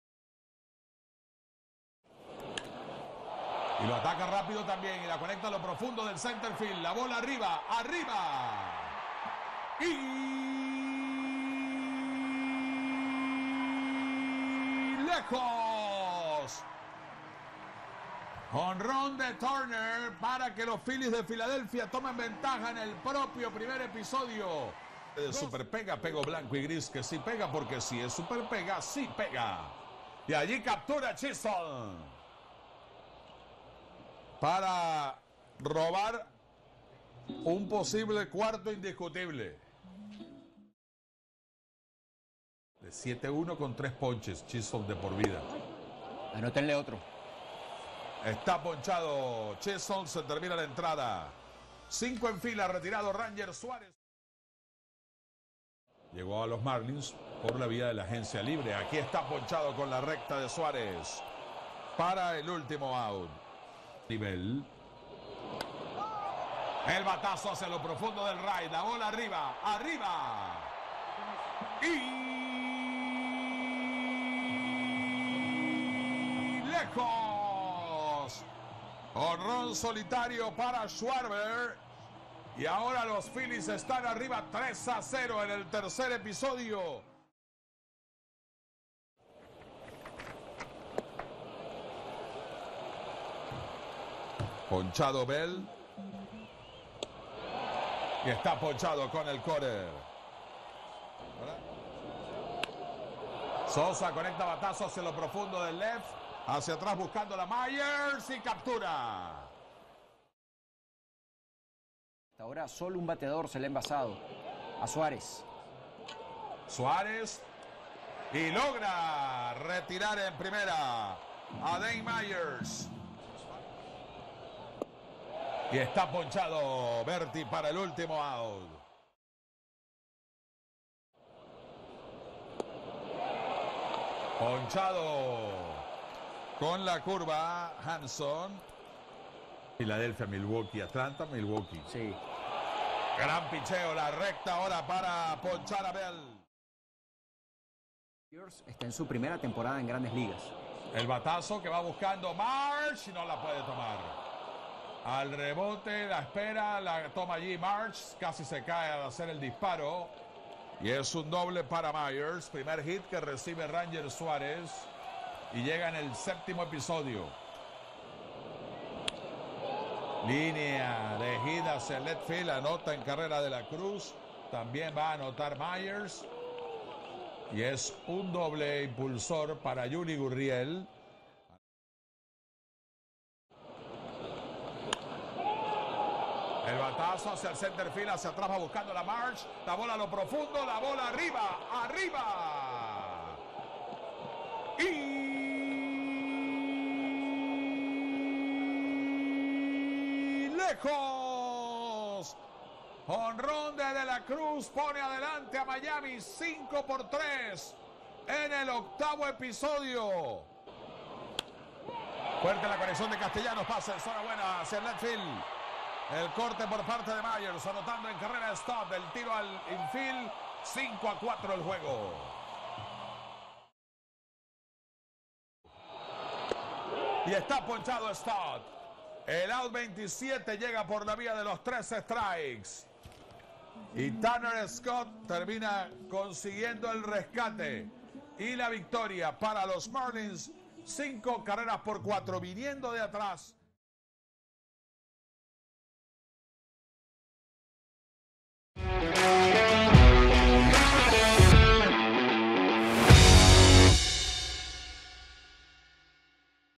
Resumen Marlins de Miami vs Filis de Filadelfia MLB 10-09-2023
Marlins de Miami dio cuenta de Filis de Filaldelfia en un disputado choque score de 5 por 4.

Por los peces sus mejores en el ataque fueron: Bryan De La Cruz al irse de 3-1, con jonrón, con dos remolcadas, Yulieski Gurriel al pegar de 3-1 con anotada, con dos fletadas y Nick Fortes al ligar de 3-1 con una producida.

🔥🔥Disfruta de la temporada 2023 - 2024 de la LVBP en vivo en la plataforma oficial de streaming del béisbol profesional venezolano.  👇👇👇